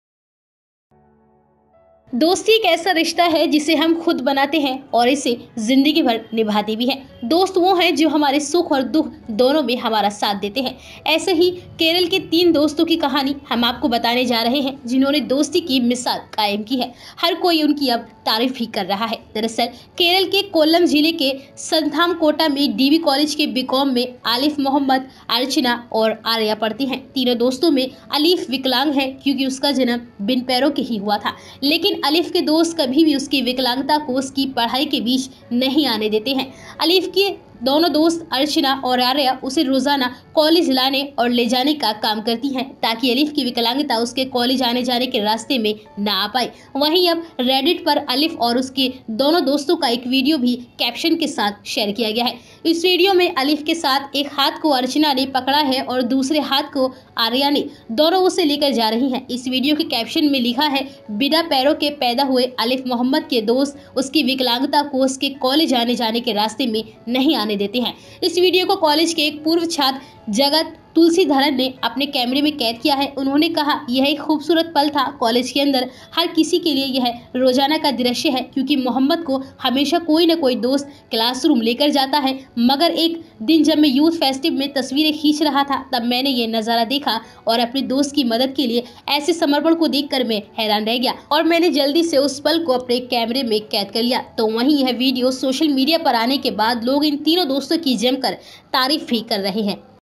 मुस्लिम दिव्यांग दोस्त को कंधो पर रोजाना कॉलेज लेकर जाती उसकी दो हिंदू दोस्त।
दोस्ती एक ऐसा रिश्ता है जिसे हम खुद बनाते हैं और इसे जिंदगीभर निभाते भी हैं. दोस्त वो हैं, जो हमारे सुख और दुख दोनों में हमारा साथ देते हैं. ऐसे ही केरल के तीन दोस्तों की कहानी हम आपको बताने जा रहे हैं जिन्होंने दोस्ती की मिसाल कायम की है हर कोई उनकी तारीफ कर रहा था
दरअसल केरल के कोल्लम जिले के सस्थामकोटा  में डीबी कॉलेज के बीकॉम में अलीफ मोहम्मद, अर्चना और आर्या पढ़ते हैं. तीनों दोस्तों में अलीफ विकलांग है क्योंकि उसका जन्म बिन पैरों के ही हुआ था लेकिन अलिफ के दोस्त कभी भी उसकी विकलांगता को उसकी पढ़ाई के बीच नहीं आने देते हैं.
अलिफ की दोनों दोस्त अर्चना और आर्या उसे रोजाना कॉलेज लाने और ले जाने का काम करती हैं. ताकि अलिफ की विकलांगता उसके कॉलेज जाने के रास्ते में ना आने पाए. वहीं अब Reddit  पर अलिफ और उनकी दोनों दोस्तों का एक वीडियो कैप्शन के साथ शेयर किया गया है.
इस वीडियो में अलिफ के एक हाथ को अर्चना ने पकड़ा है और दूसरे हाथ को आर्या ने और दोनों उसे लेकर जा रही हैं. इस वीडियो के कैप्शन में लिखा है, "बिना पैरों के पैदा हुए, अलीफ मोहम्मद के दोस्त उसकी विकलांगता को उसके कॉलेज जाने के रास्ते में नहीं आने देते."
इस वीडियो को कॉलेज में पूर्व छात्र जगत तुलसीधरन (Jagat Thulaseedharan) ने अपने कैमरे में कैद किया है. उन्होंने कहा, यह एक खूबसूरत पल था. कॉलेज के अंदर हर किसी के लिए यह रोजाना का दृश्य है क्योंकि मोहम्मद को हमेशा कोई ना कोई दोस्त क्लासरूम लेकर जाता है. मगर एक दिन जब मैं यूथ फेस्टिव में तस्वीरें खींच रहा था तब मैंने यह नजारा देखा और अपने दोस्त की मदद के लिए ऐसे समर्पण को देखकर मैं चकित था और मैंने जल्दी से उस पल को अपने कैमरे में कैद कर लिया. तो वही यह वीडियो सोशल मीडिया पर आने के बाद लोग इन दोस्तों की जमकर तारीफ करते नजर आ रहे है।